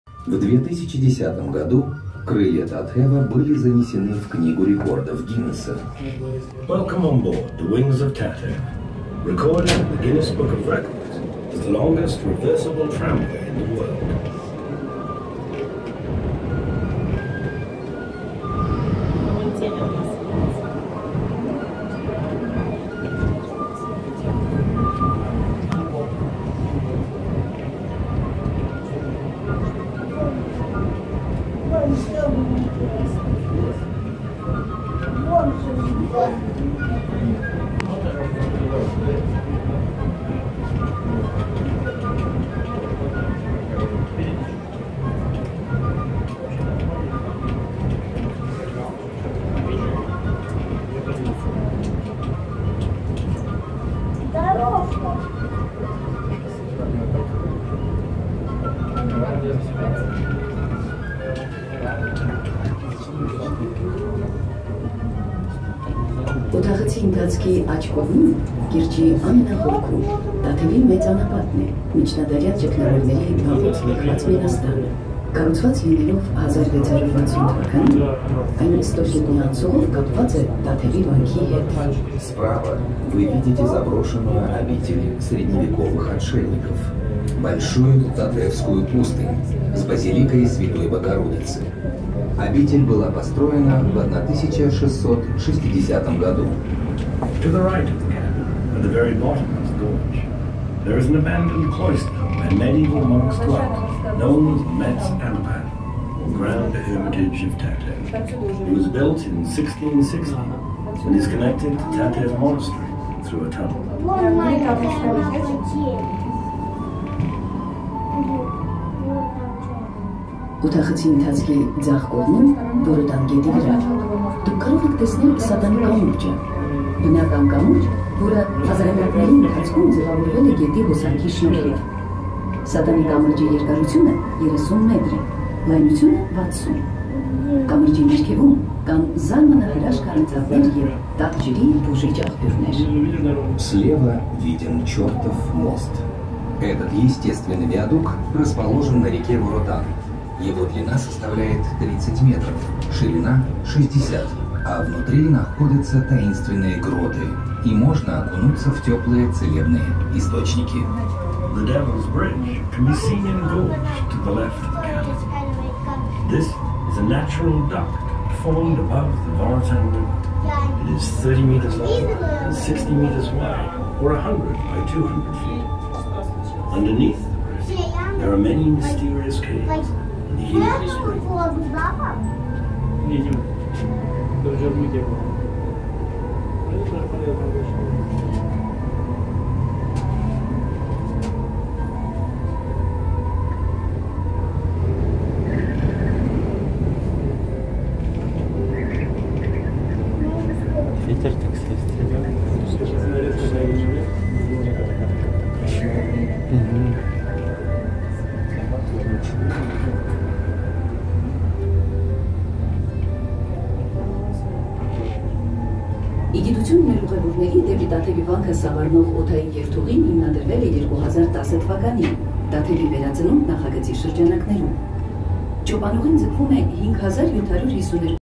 1) Захватывающее Дух Место Крылья Татева Breathtaking Place Wings Of Tatev
Канатная Дорога Крылья Татева
«Крылья Татева» являются воздушной дорогой к природным и историческим сокровищам южного региона Армении - Сюника.

«Крылья Татева» – это:
самый короткий, живописный и впечатляющий путь к Татевскому монастырю;
самая длинная реверсивная канатная дорога в мире (5752 м), попавшая в Книгу рекордов Гиннесса;
12 минут незабываемого полета над ущельем бурной реки Воротан;
небесная магистраль на высоте 320 м;
современные инженерные решения австрийско-швейцарской группы Doppelmayr/ Garaventa

Wings Of Tatev Aerial Tramway
Wings of Tatev is the aerial tramway to the natural and historic treasures of Syunik.

Wings of Tatev is:
The shortest, most picturesque and impressive route to Tatev Monastery;
The longest reversible cableway in the world (5752 m), recorded in the Guinness Book of Records.
Twelve minutes of unforgettable flight above the ravine of the fierce Vorotan River;
Celestial highway at the altitude of 320 m;
Cutting-edge engineering solutions by Austrian-Swiss Doppelmayr/ Garaventa Group.